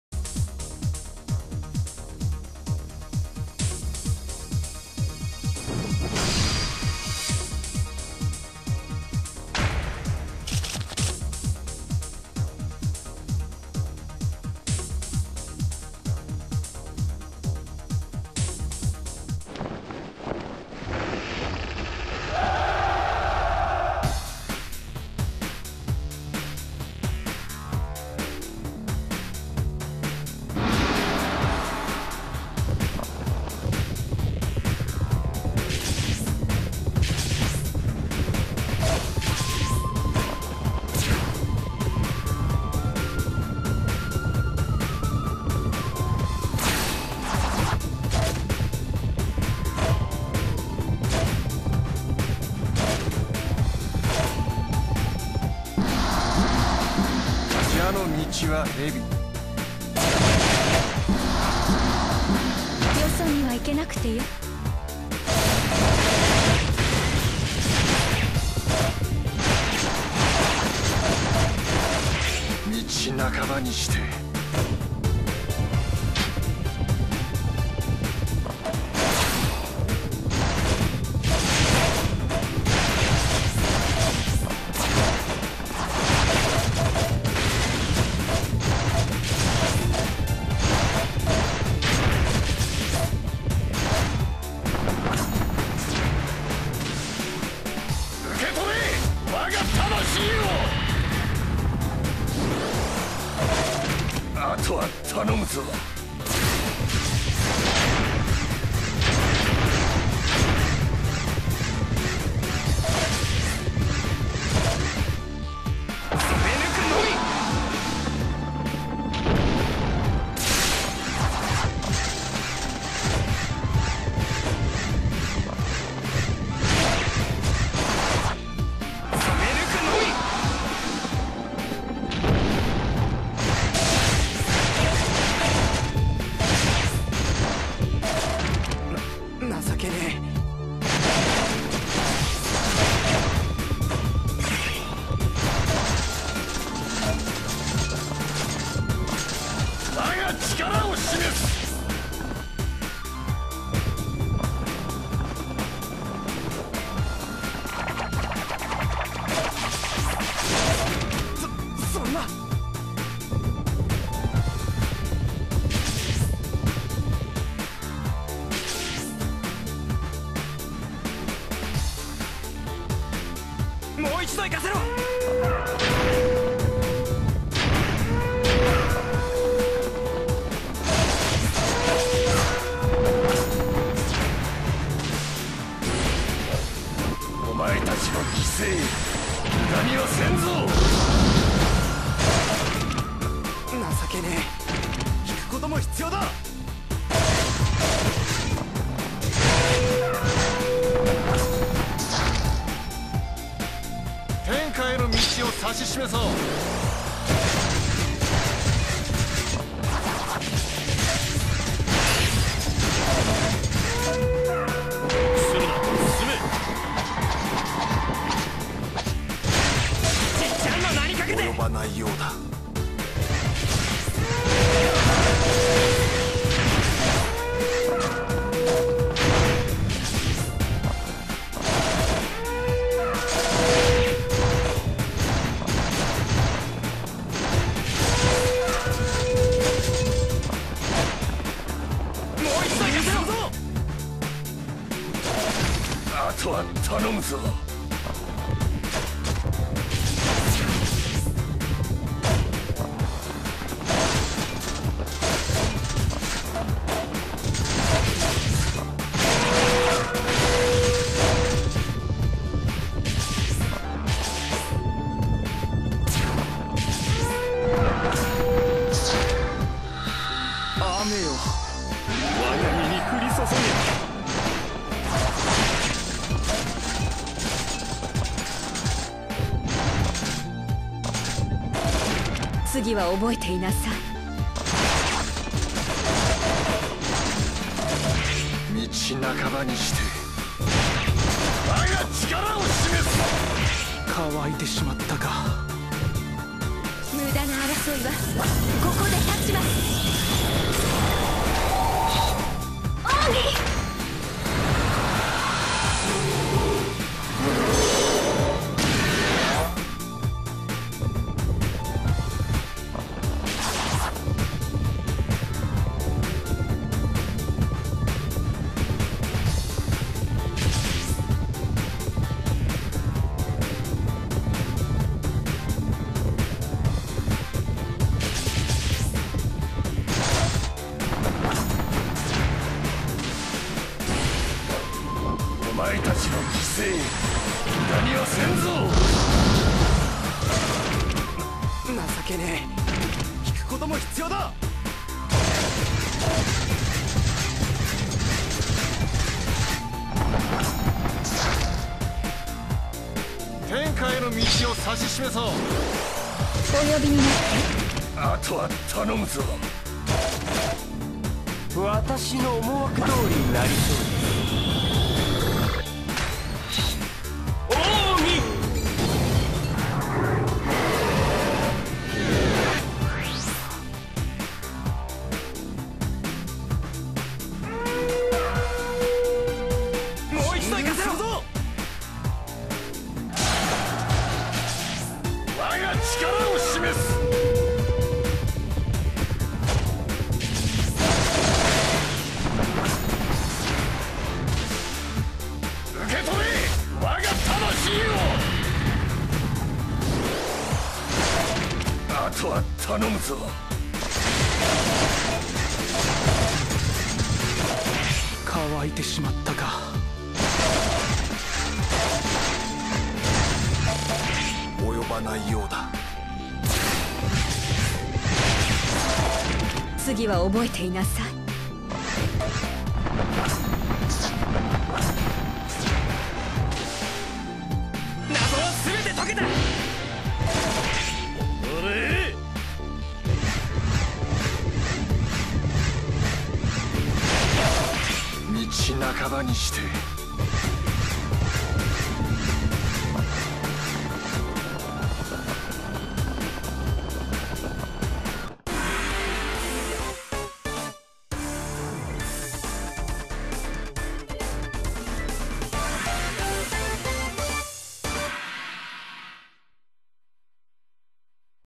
ver 3.59_1 演武場 9COST大會 魏礎雙反 vs 天人麒麟兒
魏礎雙反：SR夏侯淵、R夏侯惇、R杜預、R司馬炎、UC王元姬
天人麒麟兒：SR姜維、SR龐統、R魏延、UC吳蘭、C諸葛瞻